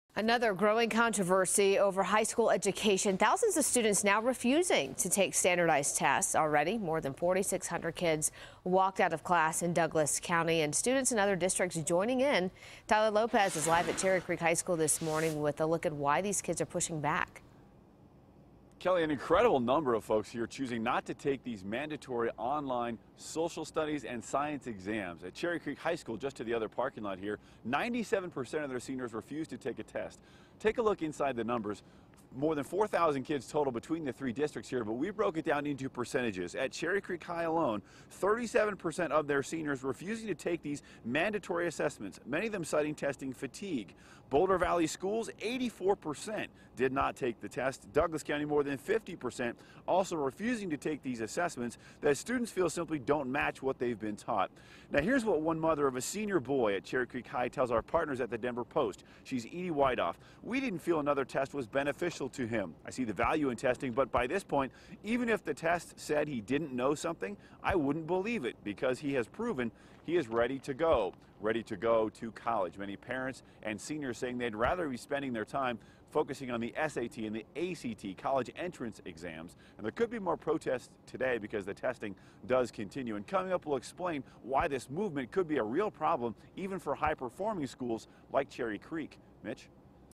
Students protest new state exam
Seniors at high schools along the Front Range are skipping a new state test that many view as pointless.